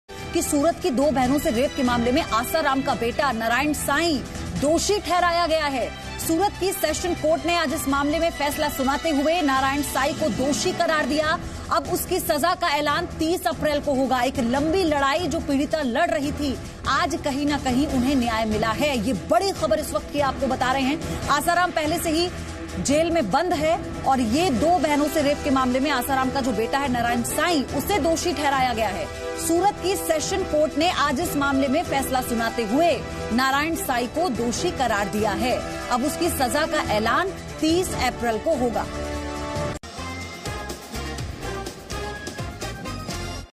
Asaram Bapu's son Narayan Sai convicted in Rape Case
Narayan Sai, son of rape convict Asaram Bapu, has been convicted by a Surat court for raping a woman. Watch this video to know more.